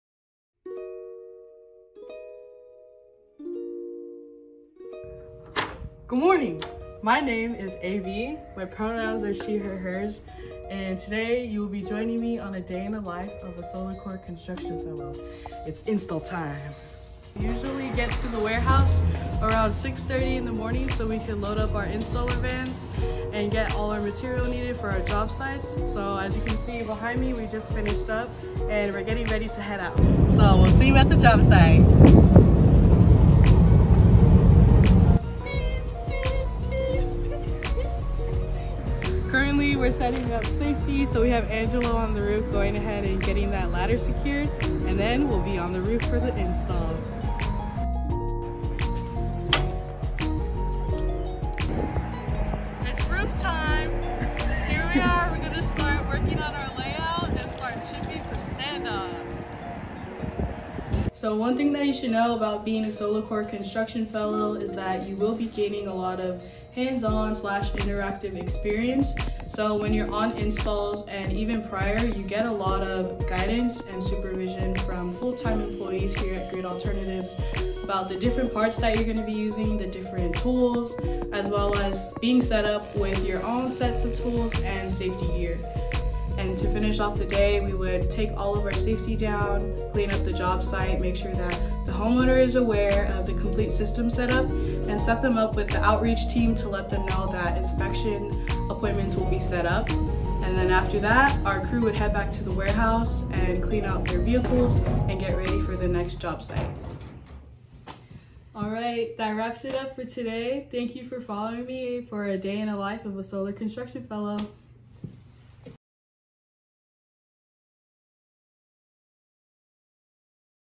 A Day in the Life: SolarCorps Construction Fellow
Get to know what a day in the life of a SolarCorps construction fellow looks like with AV. Like what you see? and apply today and check out all positions; outreach, workforce development, and more! Join GRID Alternatives in training the next cohort of clean energy experts and answering the call to an equitable transition toward a sustainable future that includes everyone!